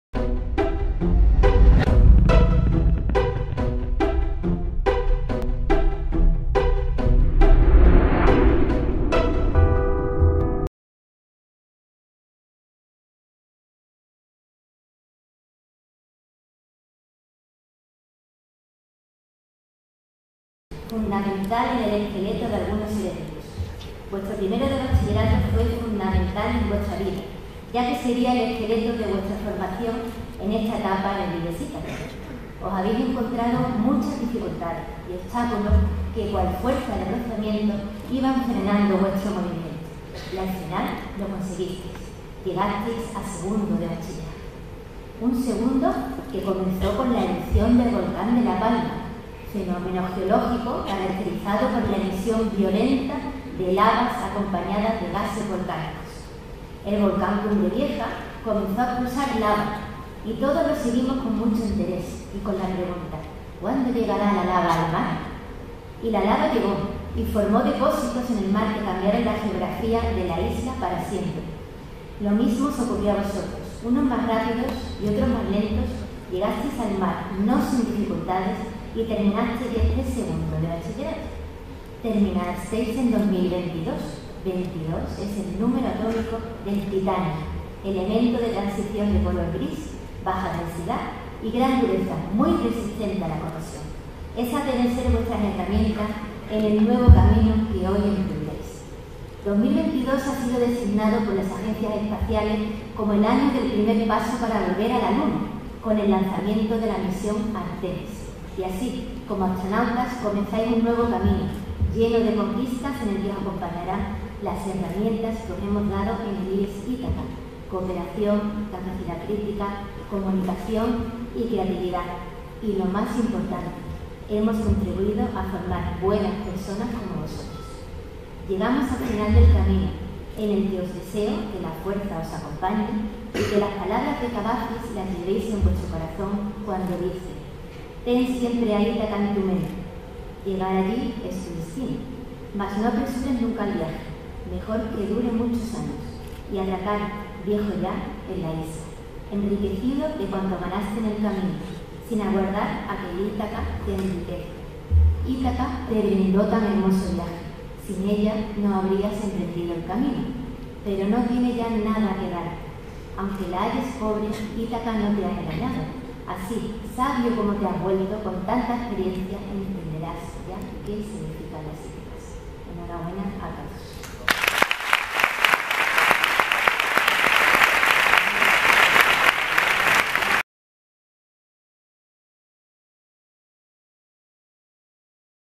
DISCURSO DE LA DIRECTORA. CAROLINA CLAVIJO. GRADUACIÓN 2º BACHILLERATO. CURSO 2021-22
El discurso de la Directora, Carolina Clavijo en la Graduación del alumnado de 2º de Bachillerato. Curso 2021-22